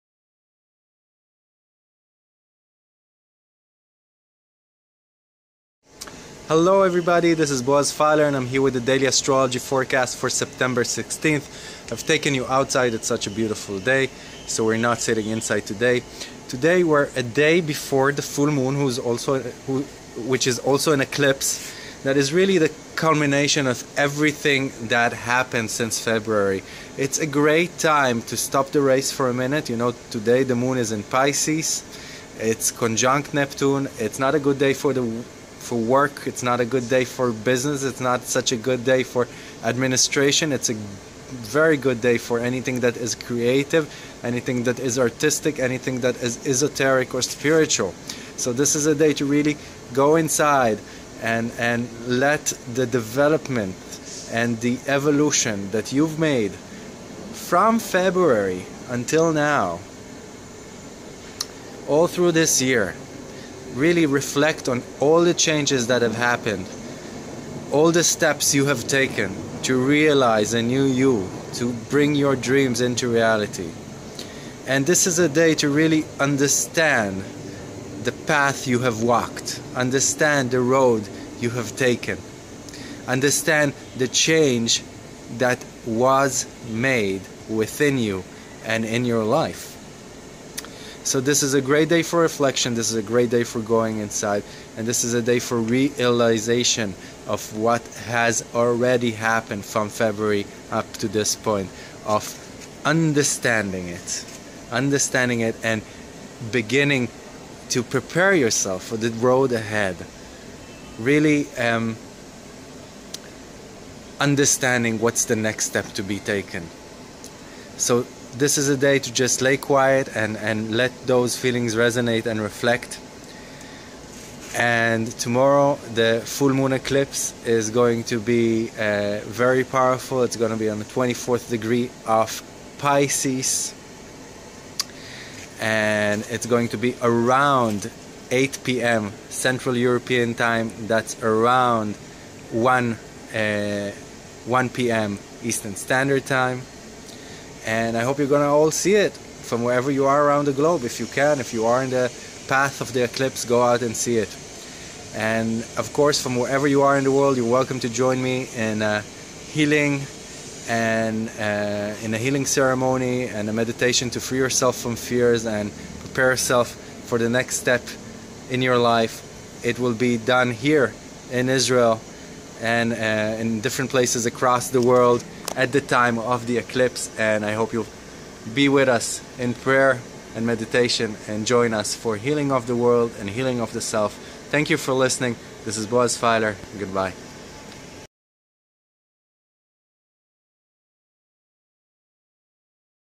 One day before the eclipse: The Daily Astrology Forecast with Boaz Fyler for 15 Sept 2016
Evolutionary Astrologer Boaz Fyler presents a practical and  concise daily astrological briefing on the celestial weather. This is your place to find a spiritual daily astrology horoscope applicable for all zodiac signs.